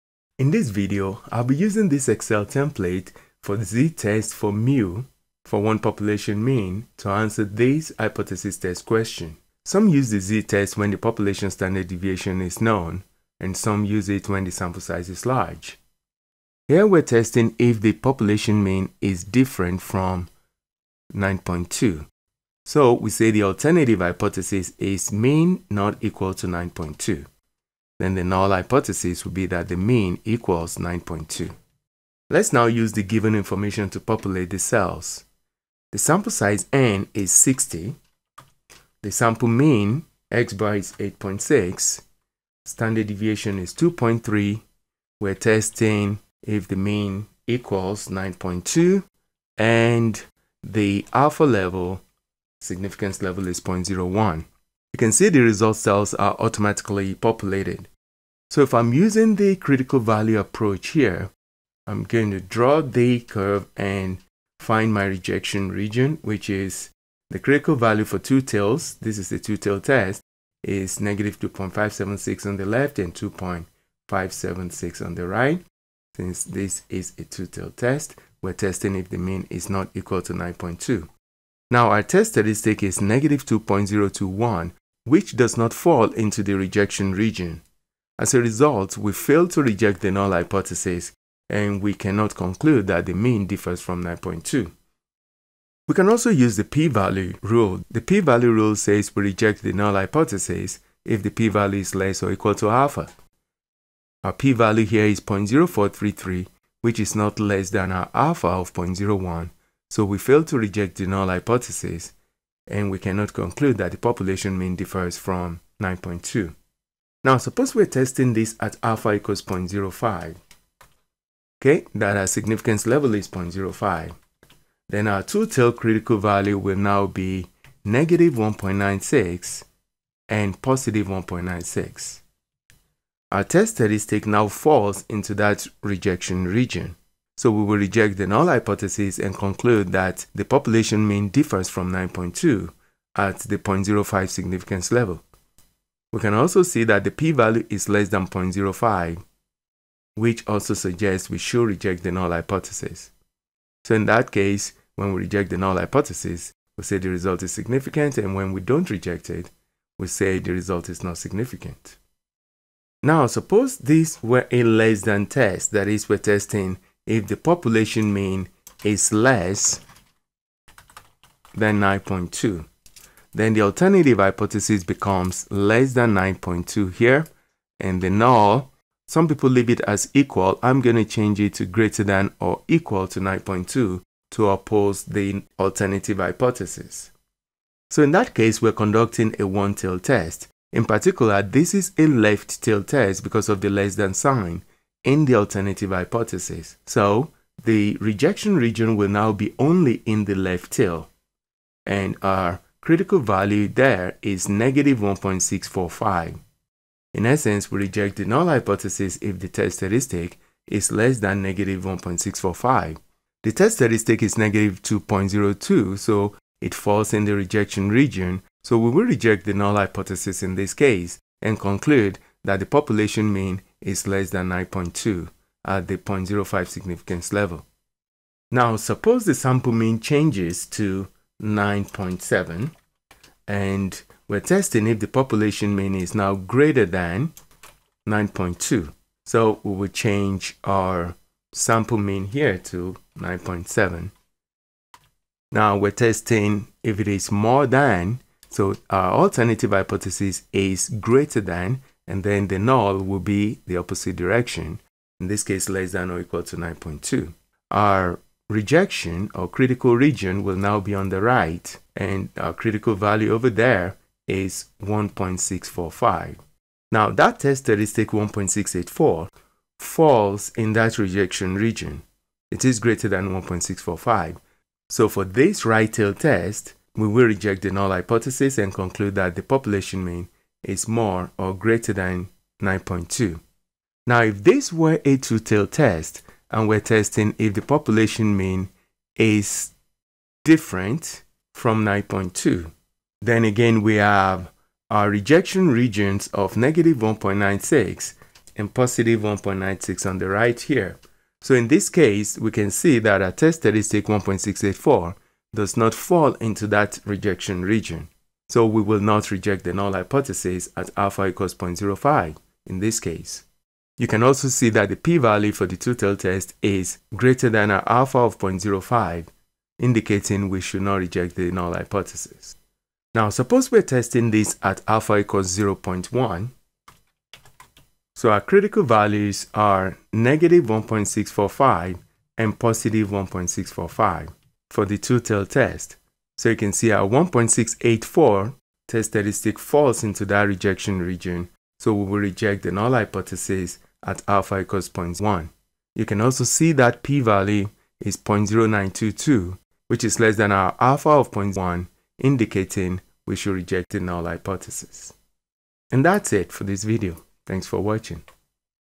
Hypothesis Z-test for mean – Excel Template | Hypothesis Testing | critical & p-values
Solved Z-test for 𝜇 – Excel Template | Hypothesis Testing | critical & p-values
This video shows various examples of how to use the Excel/Google Sheets template below to conduct significance tests for one population/sample mean, using both critical region and p-value approaches.
00:00 Example 1 (2-tailed α = 0.01)
01:57 Example 2 (two-tailed, α = 0.05)
02:42 Example 3 (left-tailed, α = 0.05)
03:48 Example 4 (right-tailed, α = 0.05)
04:44 Example 5  (two-tailed, α = 0.05, x̄ = 9.7)
05:44 Example 6  (two-tailed, α = 0.1, x̄ = 9.7)